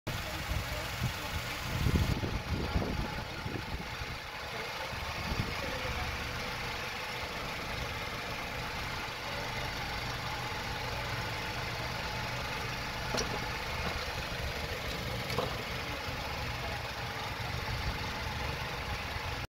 This is what an oil sump looks like when it is completely full.
working at an oil changing business, you see a lot of unusual things. this may not look too bizarre on video, but in person it really strikes the eye.